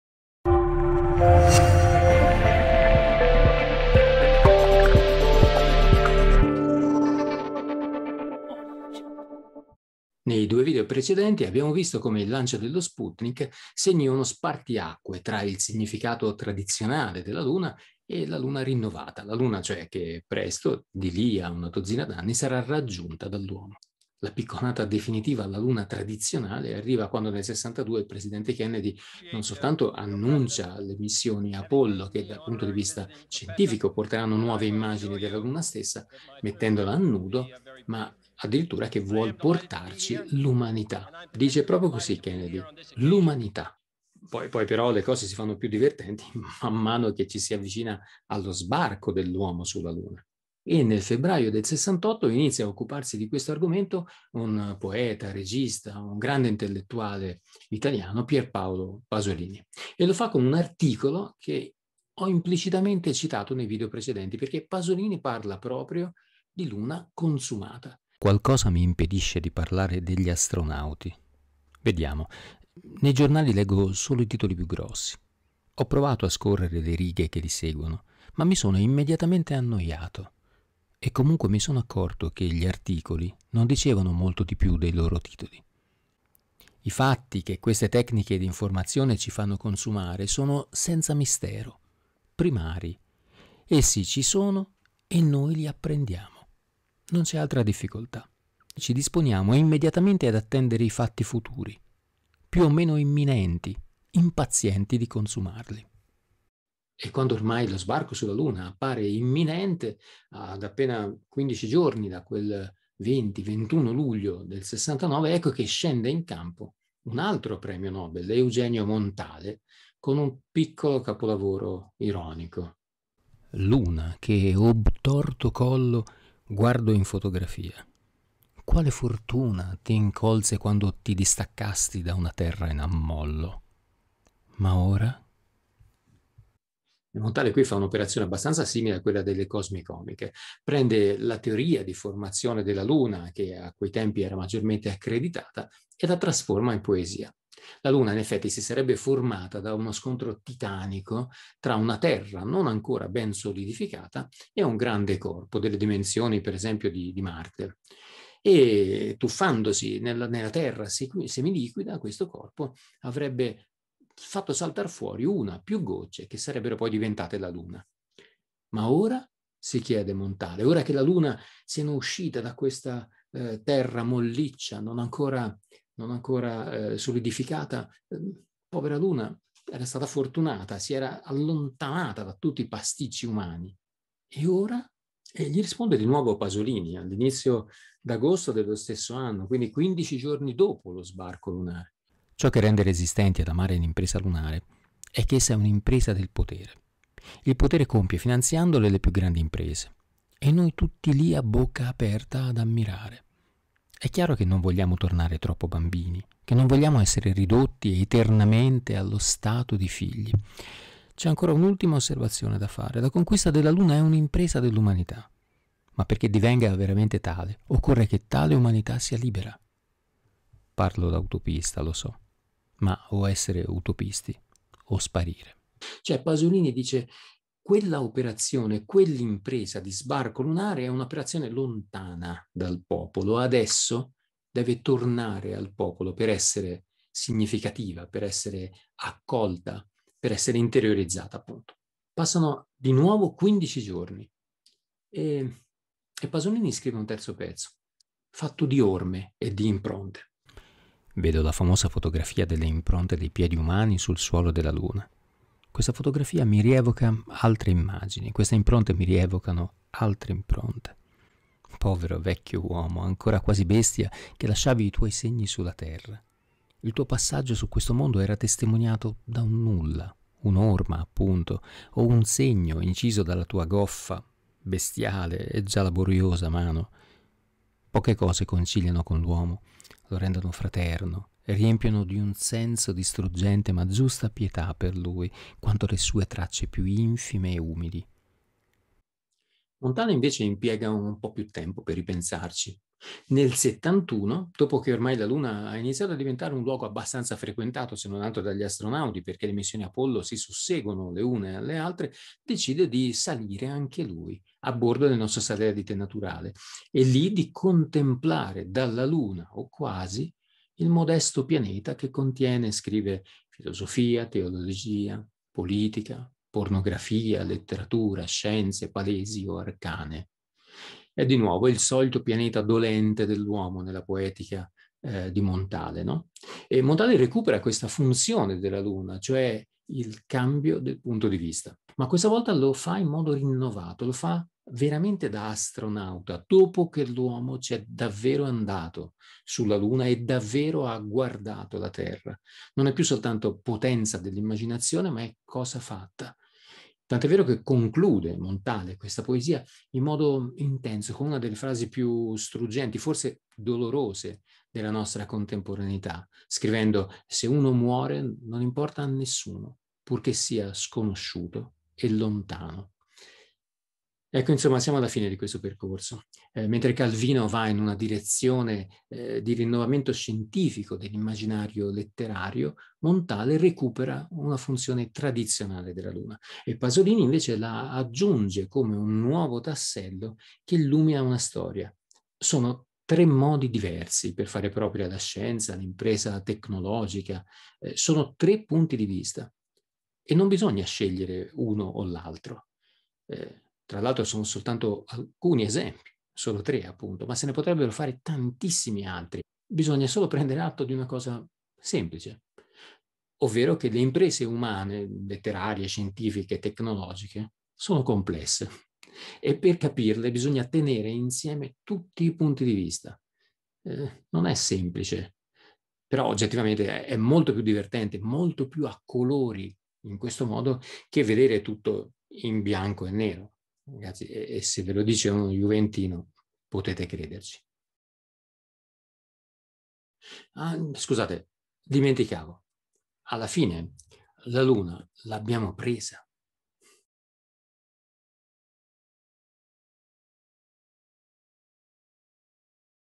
La Luna al guinzaglio
SI conclude il trittico di StefanoSandrelli dedicato alla Luna nell'arte. IN questa nuova puntata ci parla di PierPaoloPasolini ed